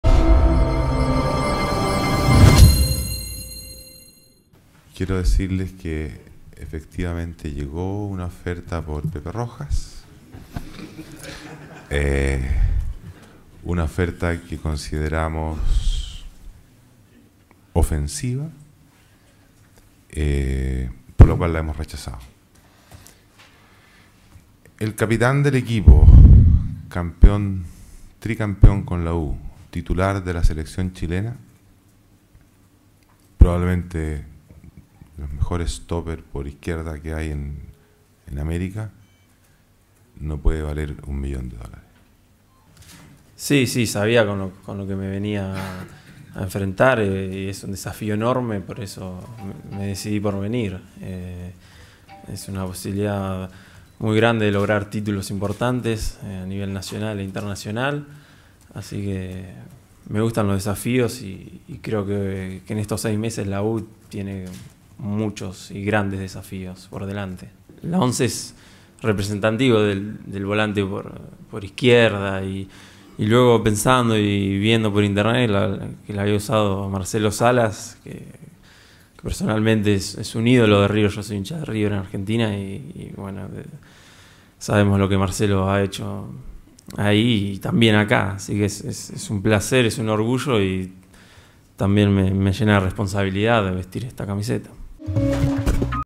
Luciano Civelli fue presentado en Universidad de Chile, la U no dejara partir a Jose Rojas|19.7.2012
Civelli fue presentado: "Me gustan los desafíos y en estos seis meses la 'U' tiene muchos"
El jugador ya conversó con Sampaoli respecto de su posición. "La idea es que juegue un poco más de adelante de lo que hacía en Libertad", comentó.